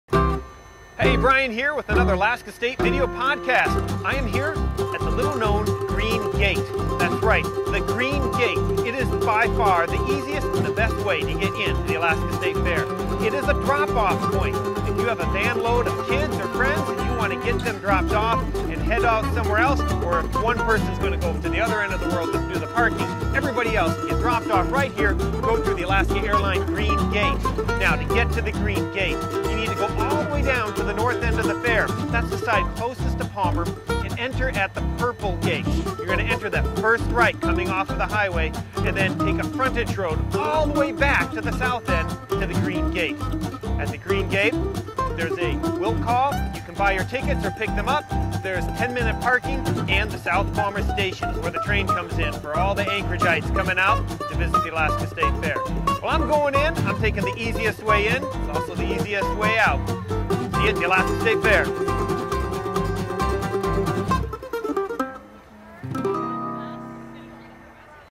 Get in to the Fair QUICK, using the Green Gate
There are little tricks to getting the most out of your experience at the Alaska State Fair.  One of them is to use the Green Gate.